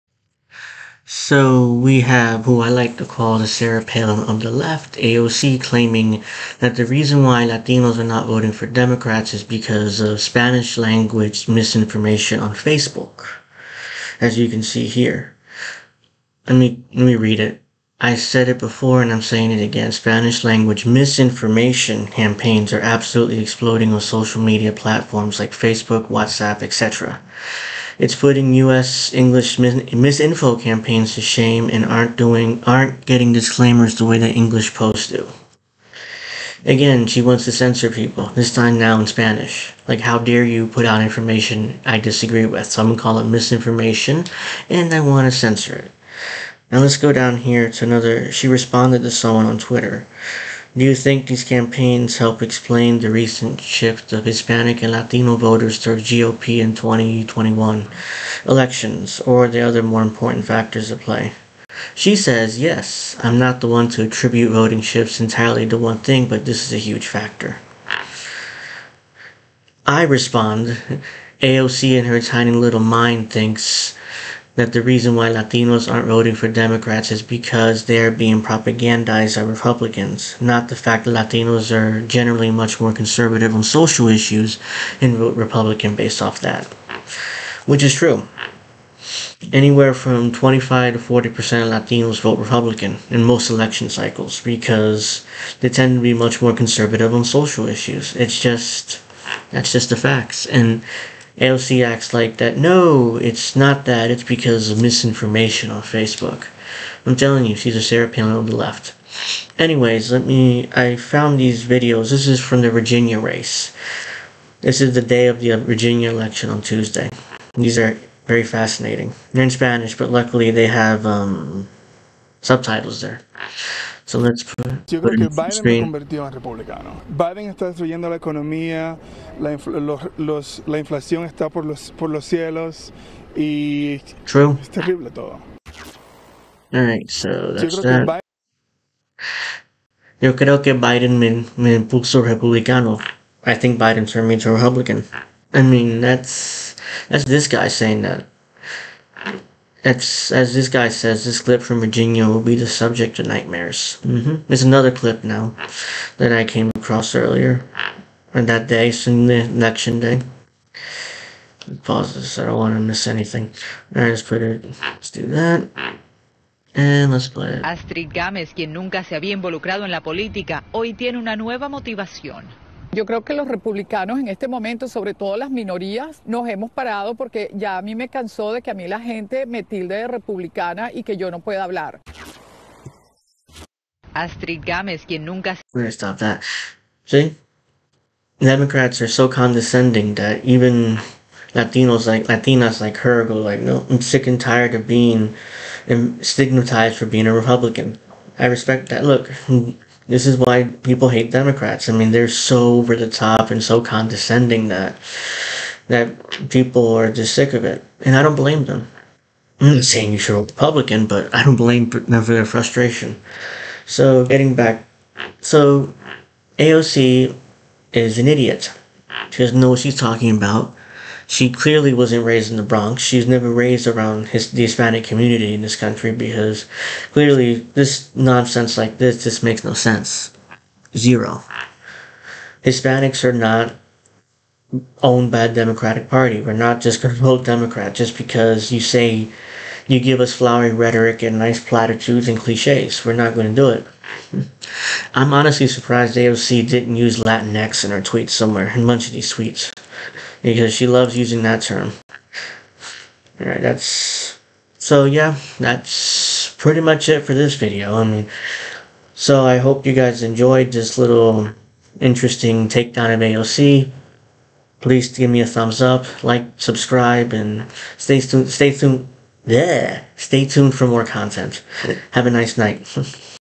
AOC thinks that Facebook should censor Spanish language "misinformation"
AOC thinks that Facebook should censor Spanish language "misinformation" and this video is my reaction to her tweets and my response to her.